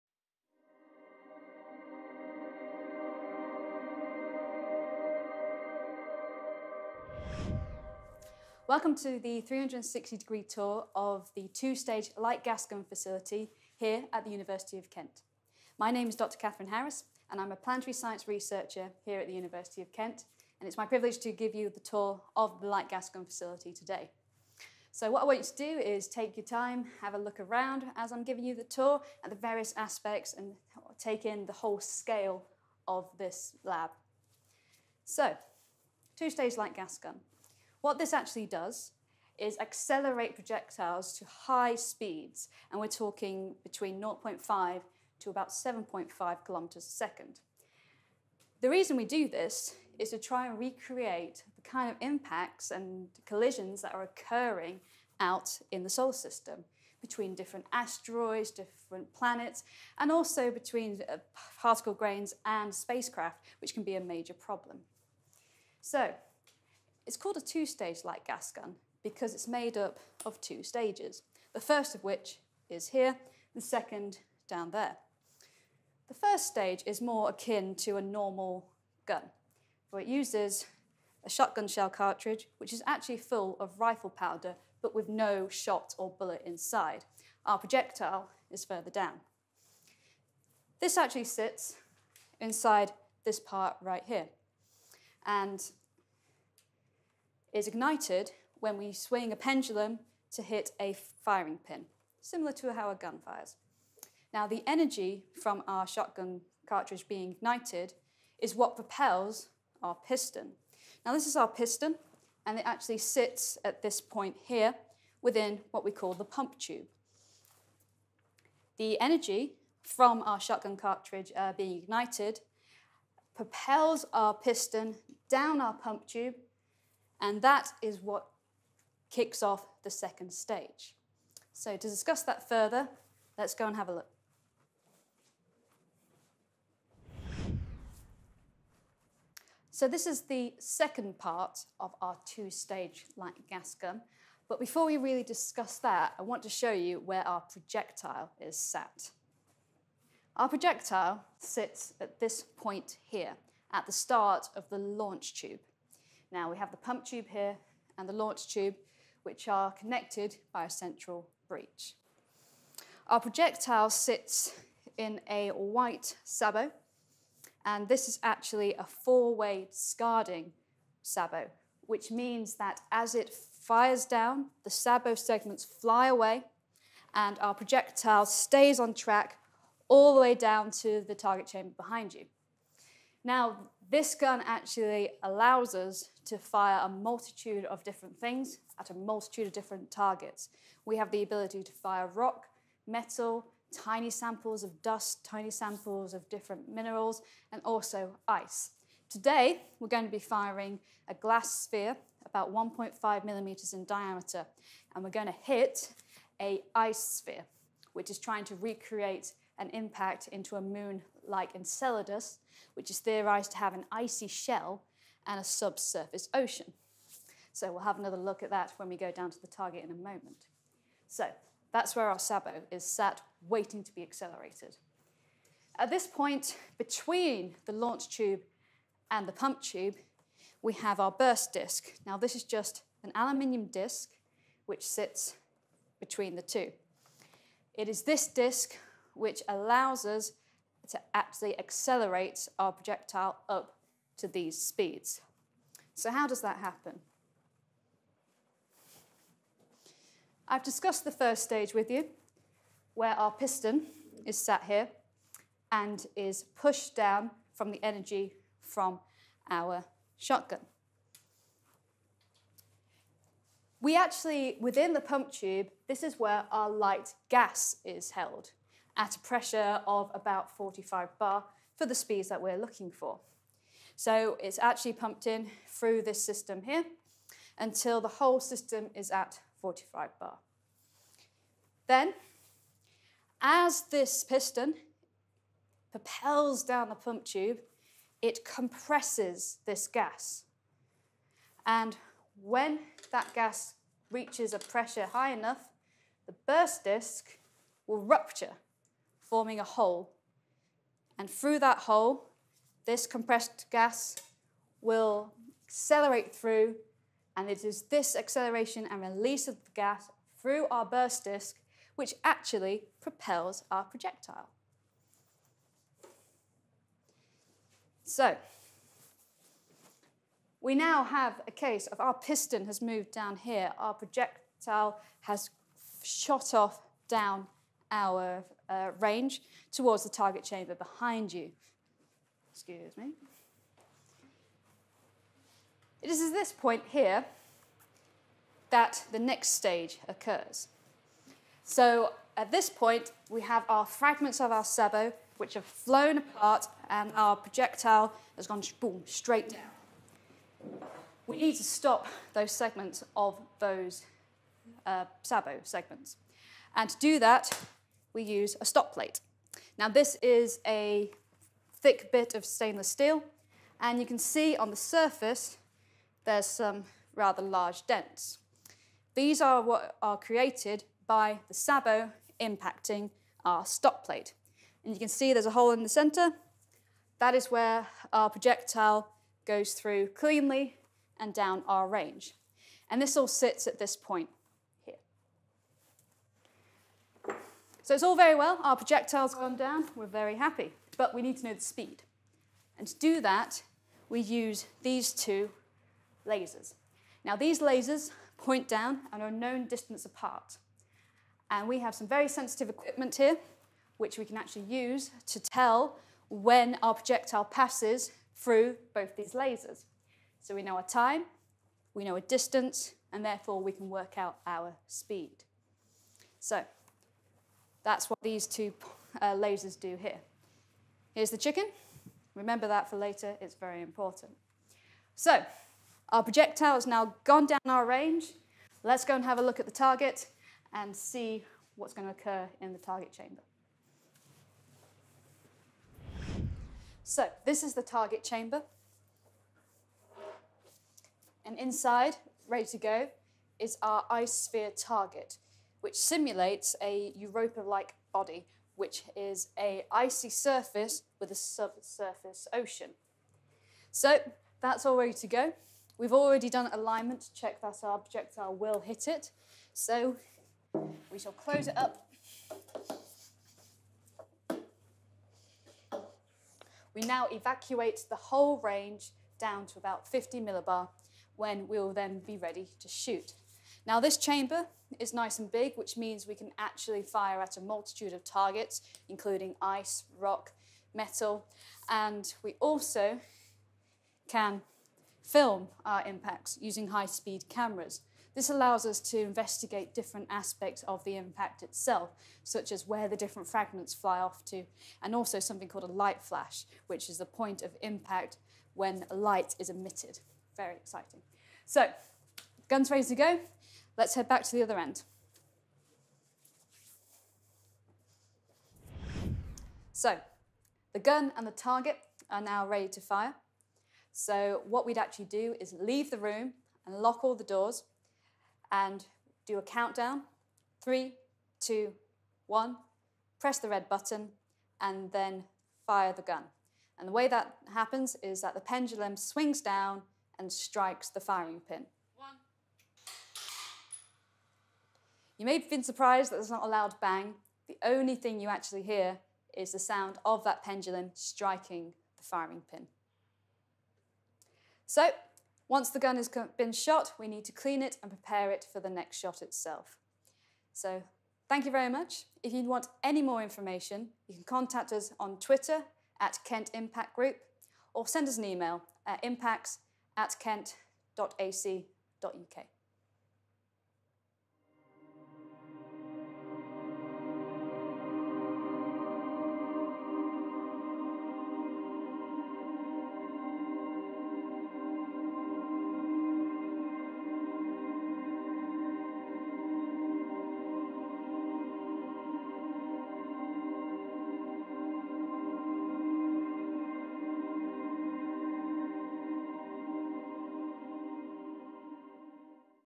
Light Gas Gun 3D 360 VR tour - University of Kent
View our exciting Light Gas Gun (LGG) with our 3D 360 VR Tour led by Dr Kathryn Harriss at the School of Physical Sciences, University of Kent.

The Light Gas Gun is used for investigations of hypervelocity impact phenomena, and is a fantastic facility for the Centre for Astrophysics and Planetary Science at Kent.

Click and drag your cursor to look around on a computer screen, or view in 3D via a Virtual Reality headset.